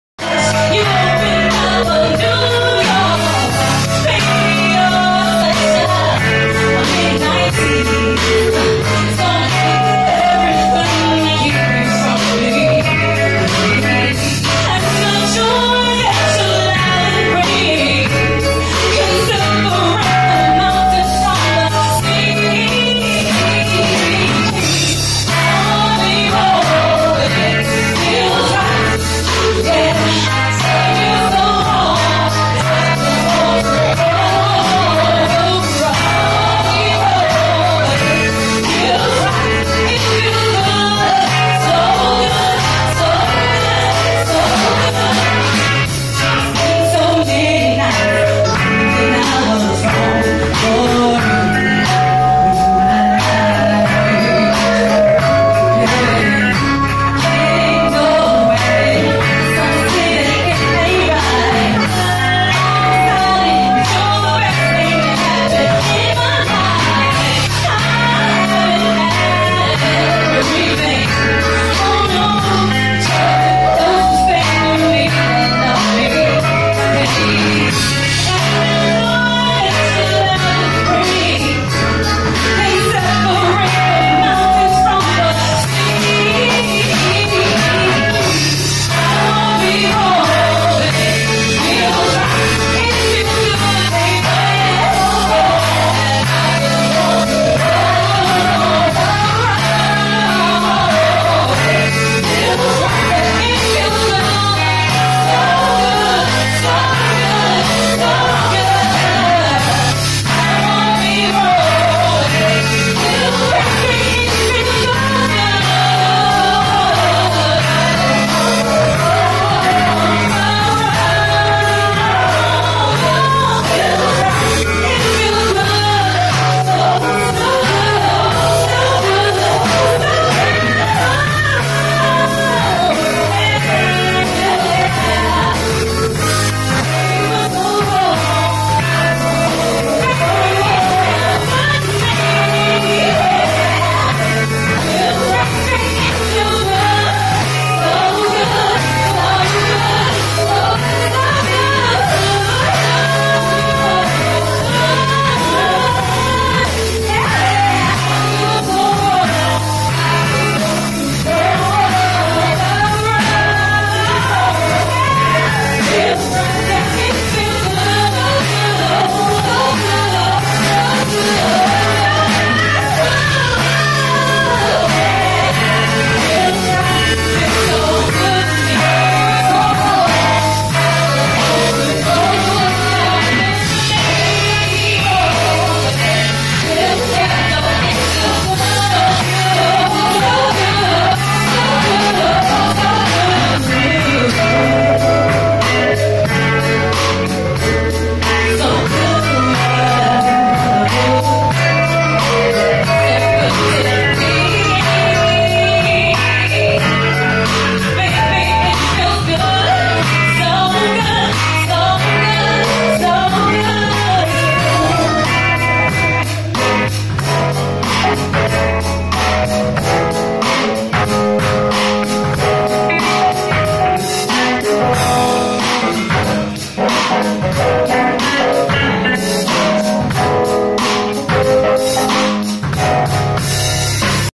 Vision Quest @ Infinity Grooming Loft Grand Opening
Vision Quest band performing at the grand opening of Infinity Grooming Loft in downtown Atlanta on 4-8-18